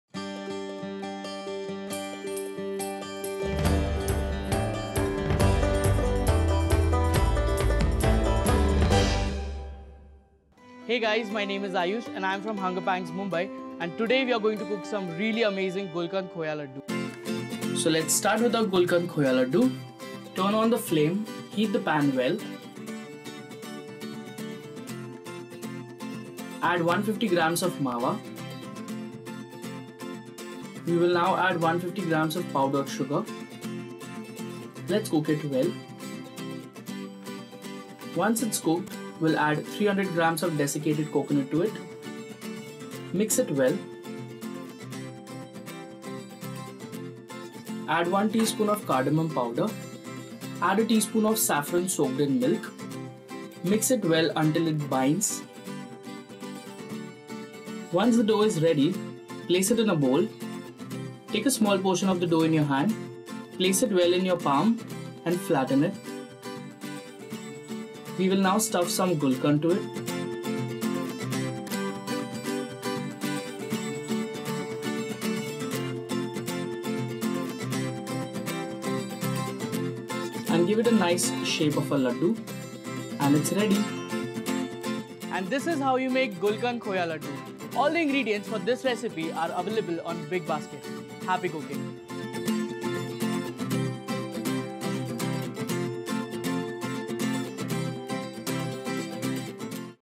Gulkand Khoya Ladoo
Gulkand Khoya Ladoo is one of family favorite cause it's just so delicious and easy to prepare.